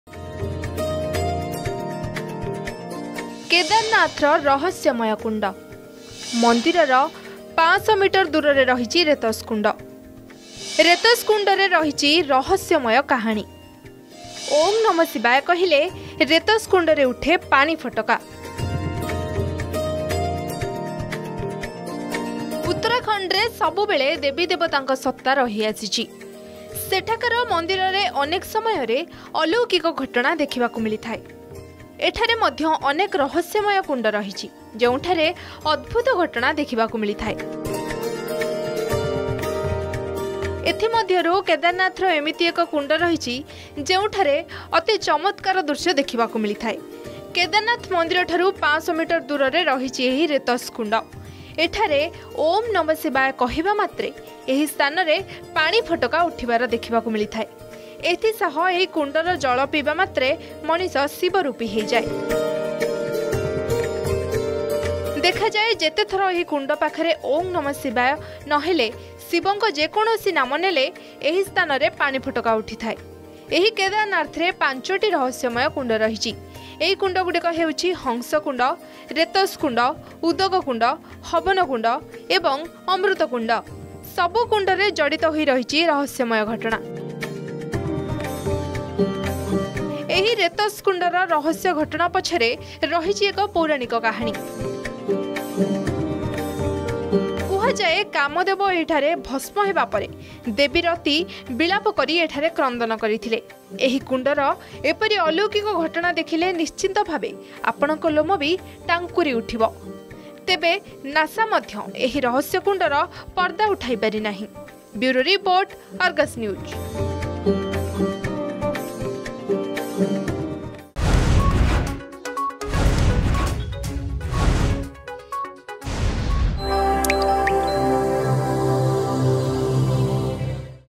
ଓମ୍ ନମଃ ଶିବାୟଃ ଜପ କଲେ ଉଠିଥାଏ ପାଣି ଫୋଟକା
ଉତ୍ତରାଖଣ୍ଡରେ ସବୁବେଳେ ଦେବୀ, ଦେବତାଙ୍କ ସତ୍ତା ରହିଆସିଛି । ସେଠାକାର ମନ୍ଦିରରେ ଅନେକ ସମୟରେ ଅଲୌକିକ ଘଟଣା ଦେଖିବାକୁ ମିଳିଥାଏ । ଏଠାରେ ମଧ୍ୟ ଅନେକ ରହସ୍ୟମୟ କୁଣ୍ଡ ରହିଛି ଯେଉଁଠାରେ ଅଦ୍ଭୁତ ଘଟଣା ଦେଖିବାକୁ ମିଳିଥାଏ । ଏଥିମଧ୍ୟରୁ କେଦାରନାଥରେ ମଧ୍ୟ ଏମିତି ଏକ କୁଣ୍ଡ ରହିଛି ଯେଉଁଠି ଏକ ଚମତ୍କାର ଦେଖିବାକୁ ମିଳିଥାଏ ।